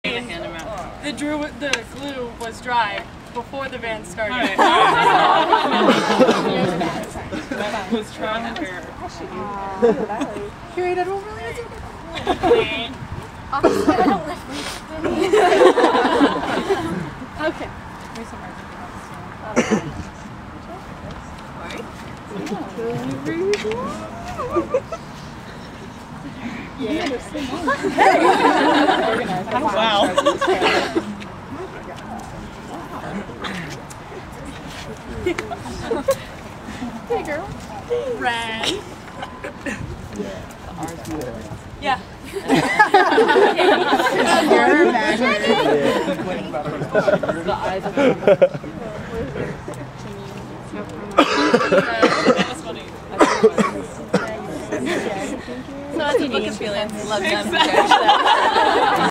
May 5 2010 064 Senior Books.MOD
The juniors handing out the senior scrapbooks after our team dinner at Mama Lucia's in Potsdam on Wednesday, May 5th, 2010.